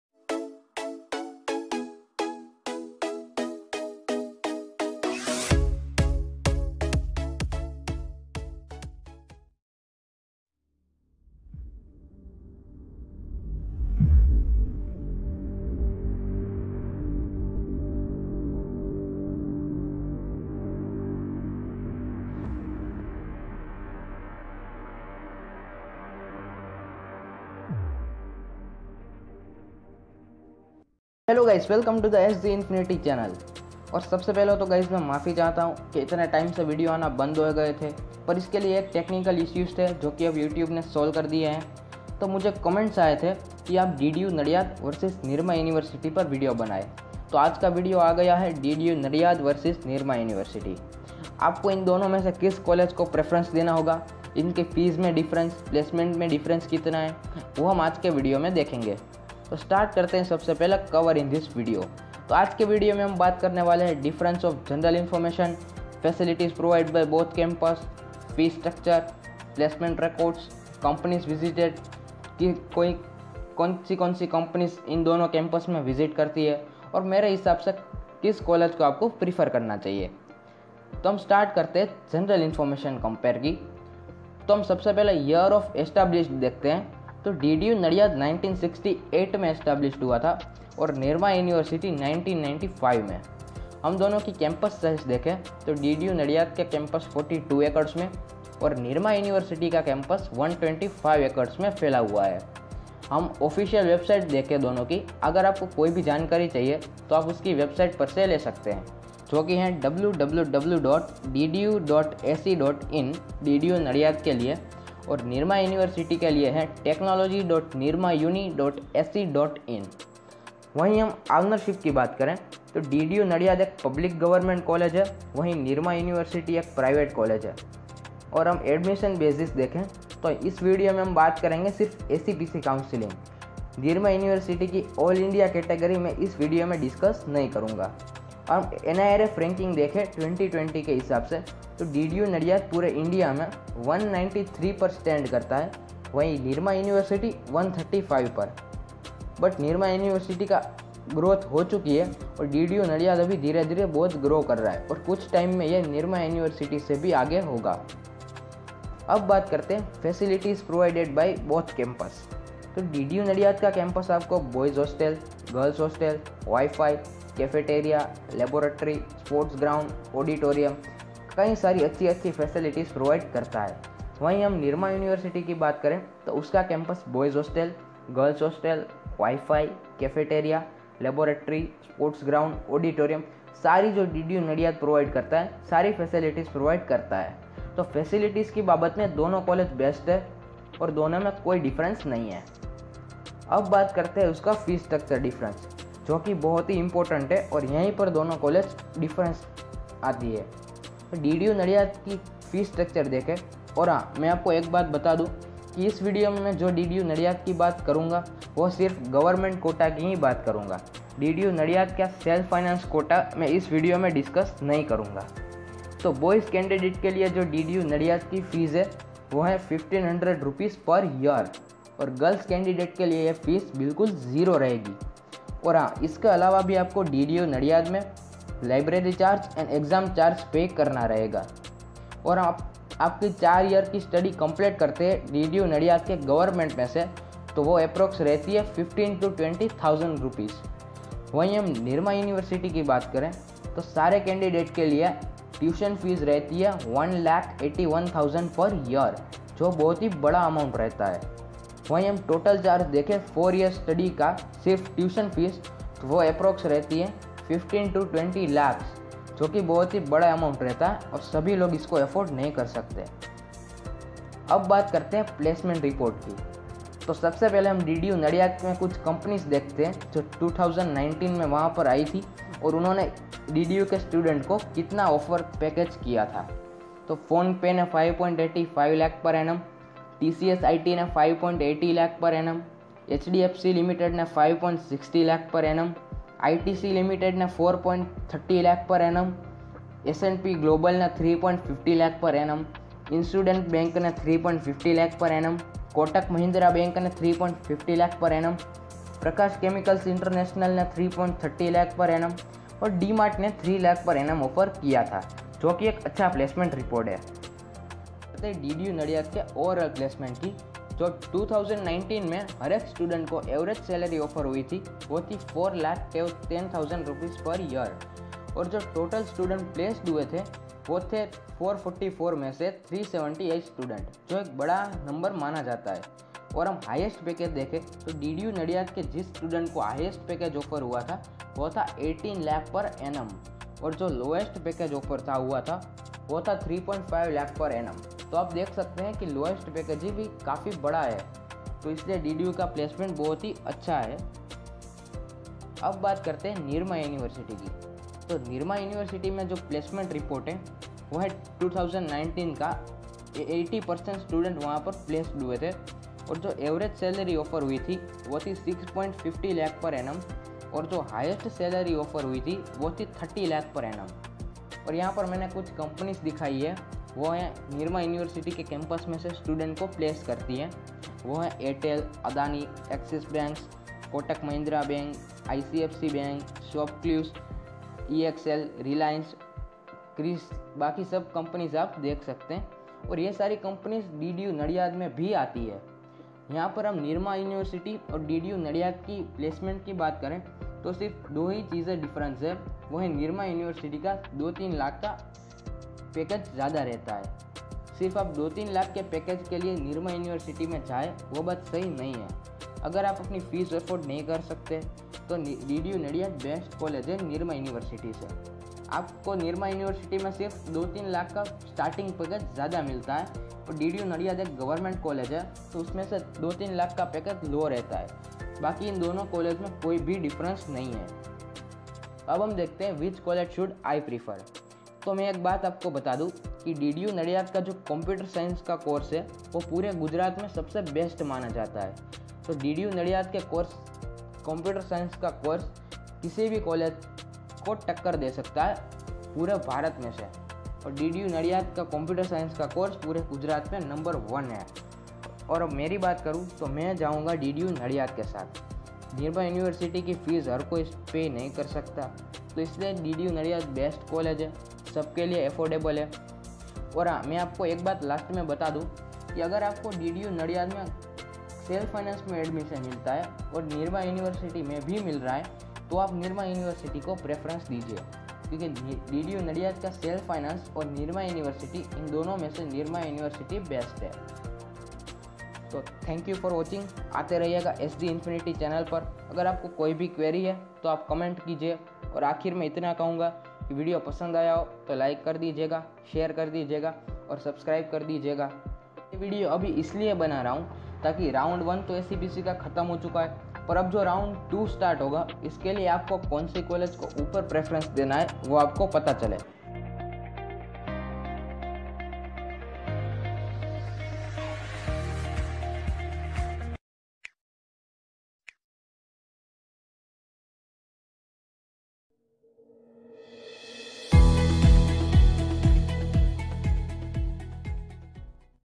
DDU NADIAD VS NIRMA UNIVERSITY COMPARISON....
Hi guys, acpc counseling will start at 21 september. So i will cover top collages in gujrat. So today i uplod DDU NADIAD VS NIRMA UNIVERSITY.These are top collages of gujrat. In this video i will cover differnce and comparision of ddu nadiad and nirma university.This video is on reality of privet vs government collage...

ddu nadiad
nirma university

also watch